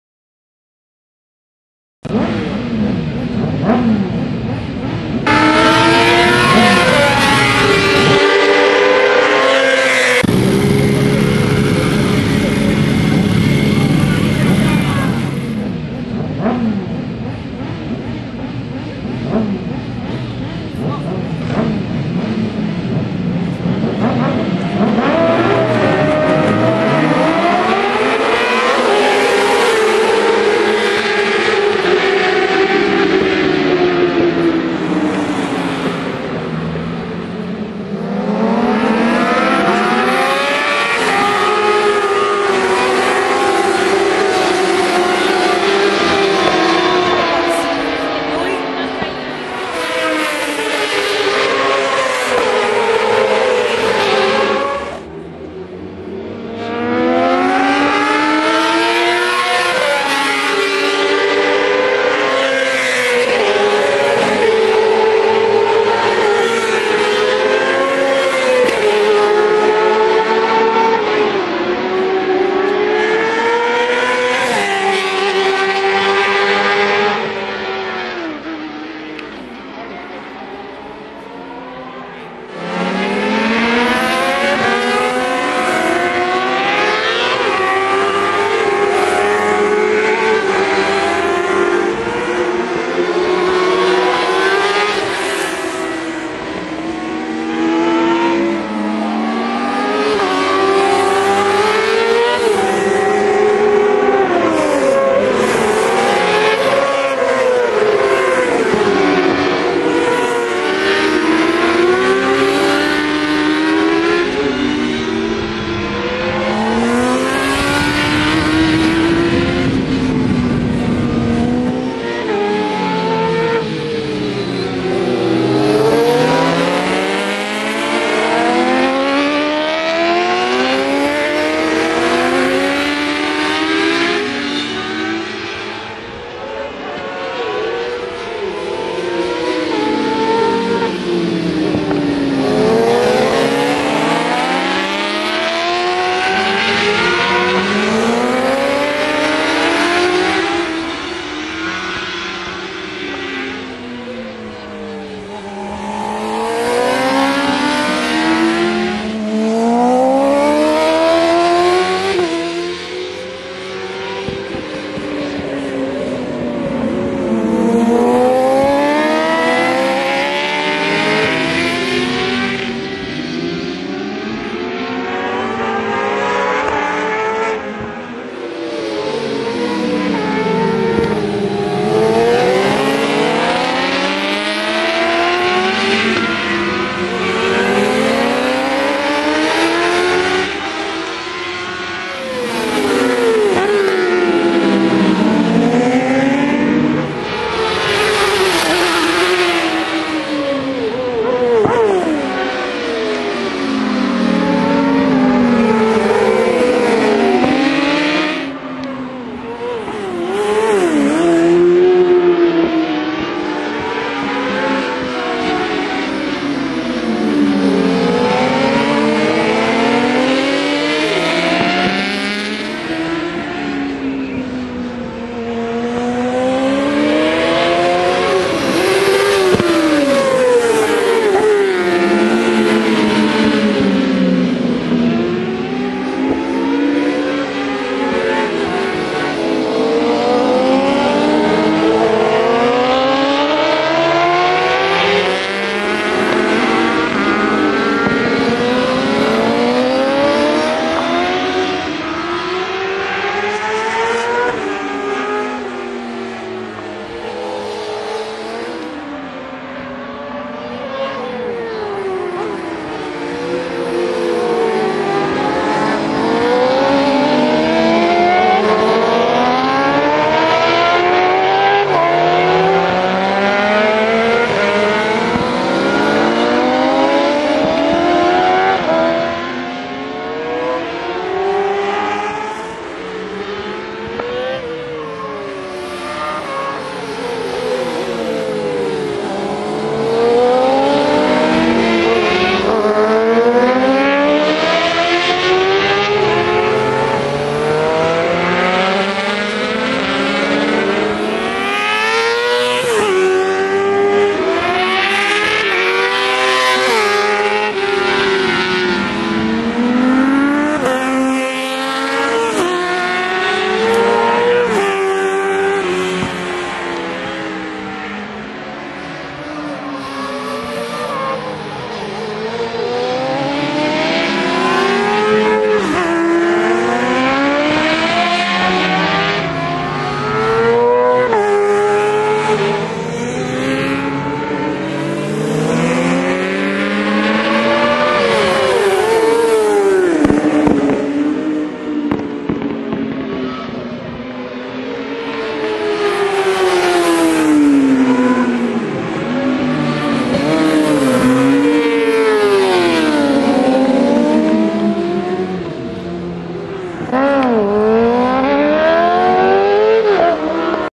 600 Rookies Trofeo Inverno 2015 - Gara 3
Automania - Classe 600 Rookies al Trofeo Inverno 2015 - Gara 3 gare motoristiche all'Autodromo del Levante sul circuito di Binetto vince Massimo Liscio